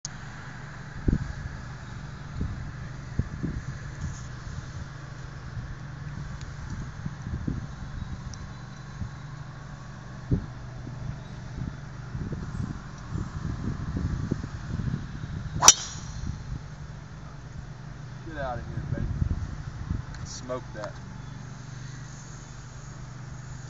Danny crushes the ball at Montgomery Bell State Park
Danny (my favorite son) and I were spending a couple of nights at Montgomery Bell State Park near Dickson, TN.  We played 27 holes of golf at the state park course.  Danny was crushing his drives...This one went around 315 yards!!